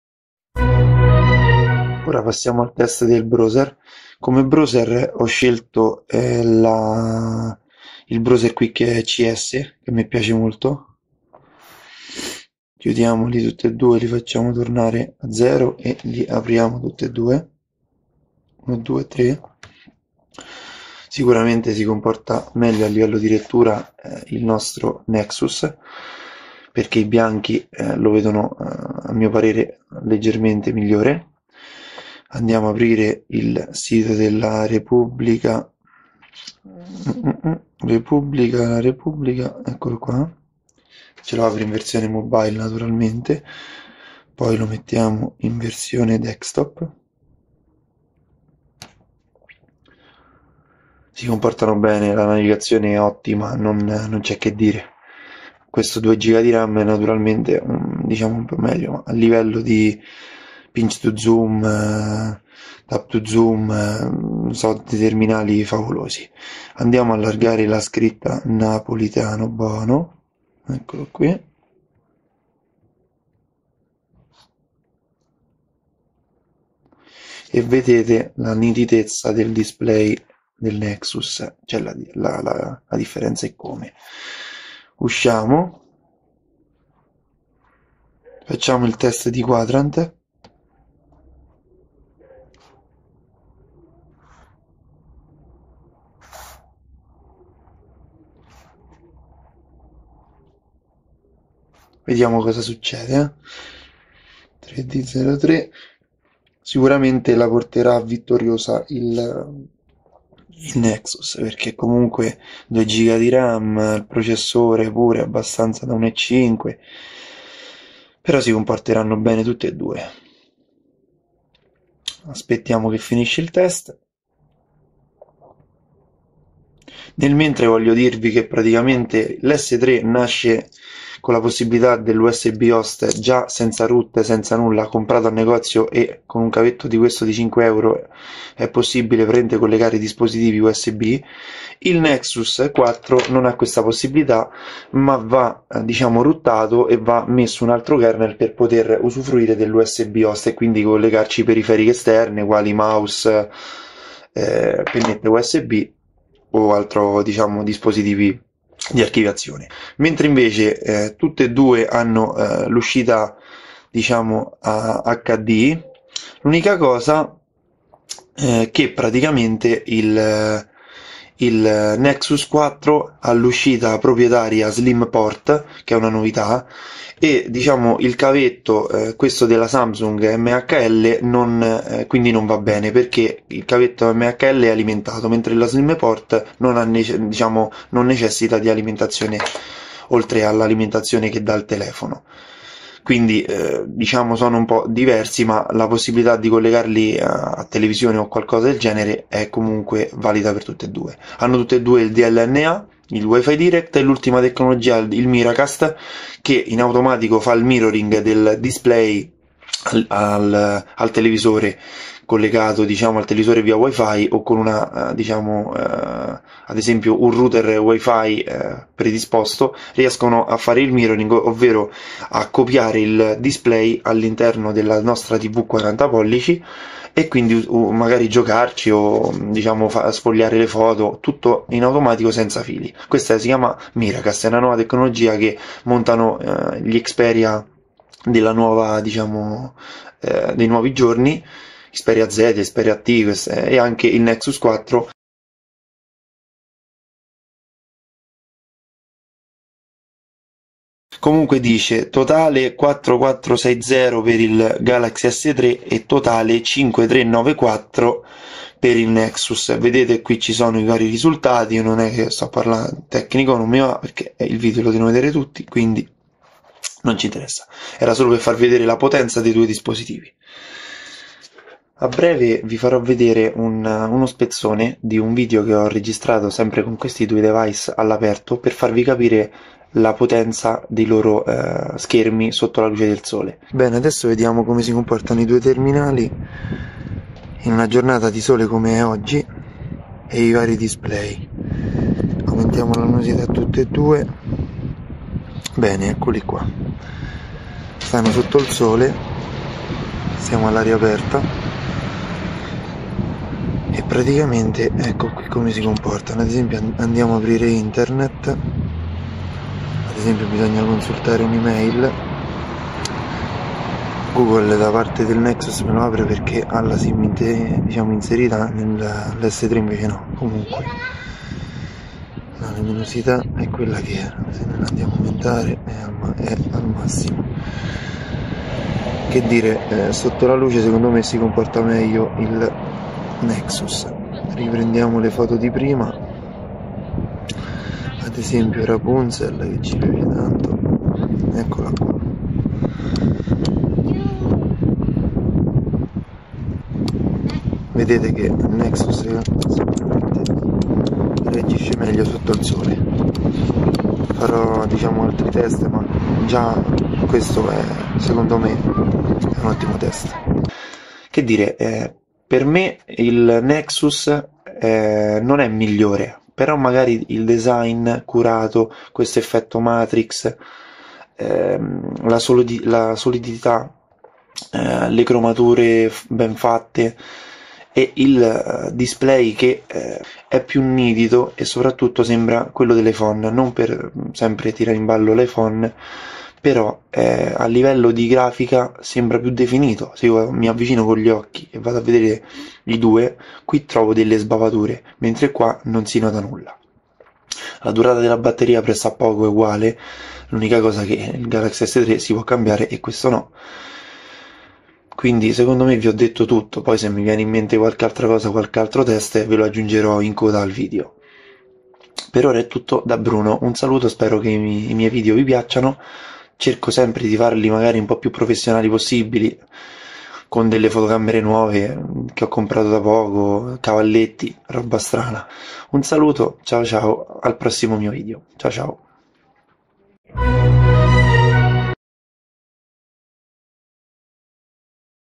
galaxy s3 vs nexus 4
galaxy s3 vs nexus 4 - PARTE 2
Impressioni, test, osservazioni e sfida tra il samsung galaxy s3 e LG nexus 4 .
PARTE 1
Test fotocamere
Test registrazione video a 1080p
Test display con wallpaper scaricati
Test riproduzione video HD
PARTE 2
Test browser
Test quadrant
Test video dei due device sotto la luce del sole.

Conclusioni